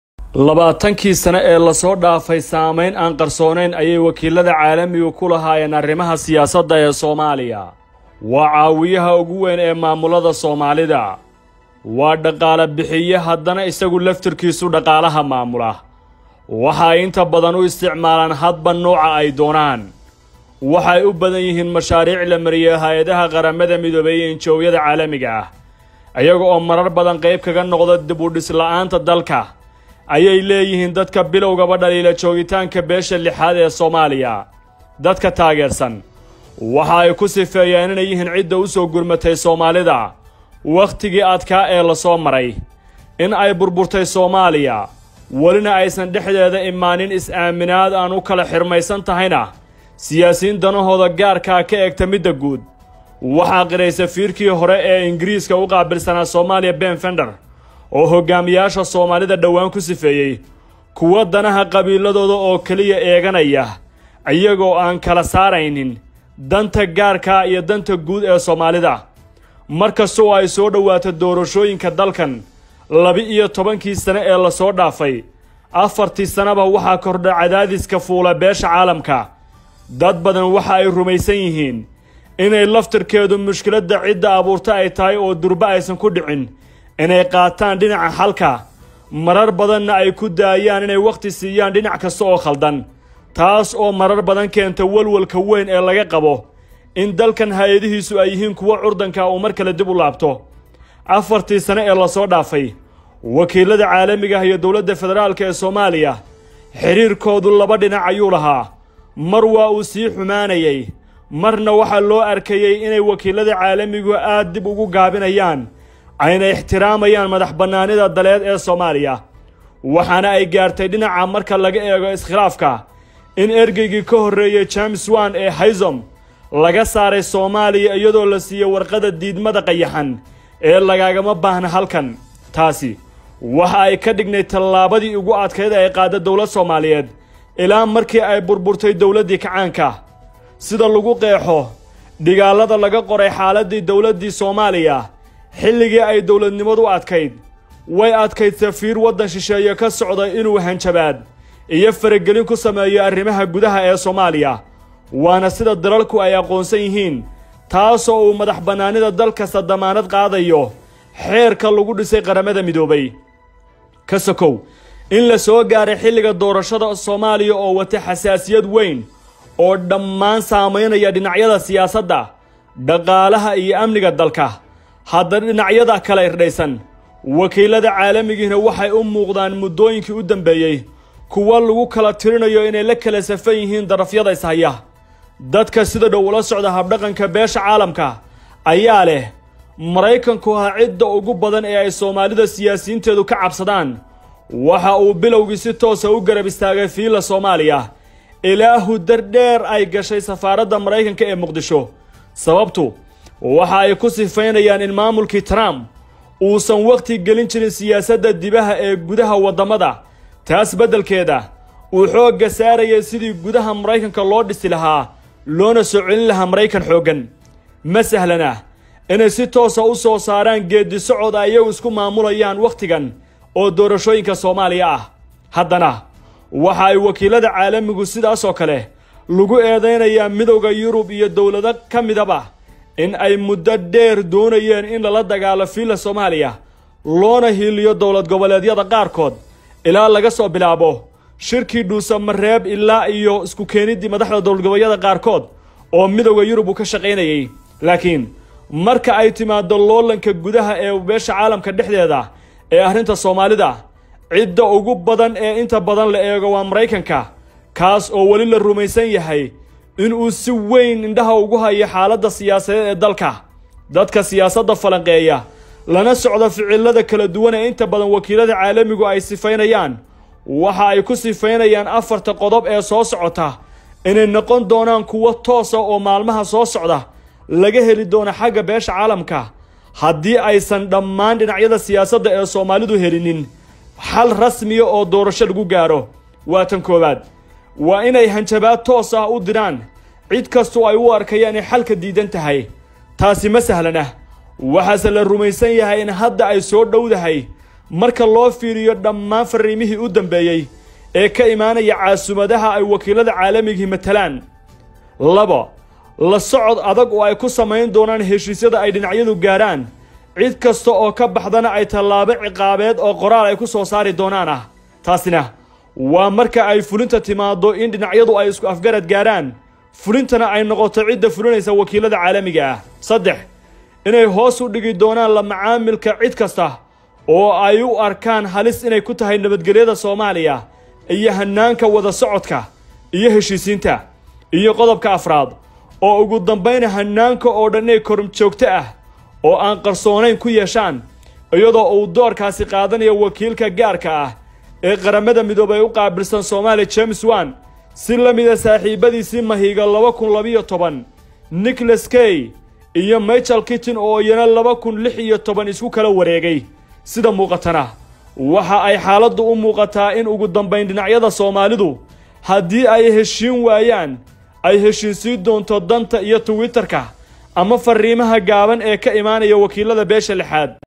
Hoggaamiyayaasha Soomaalida iyo Beesha Caalamka, maxaa kala haysta, Yaa leh Talada Xiligan,.
Hoggaamiyayaasha Soomaalida iyo Beesha Caalamka, maxaa kala haysta, Yaa leh Talada Xiligan, Miyey kala qeybsan tahay Beesha 6aad?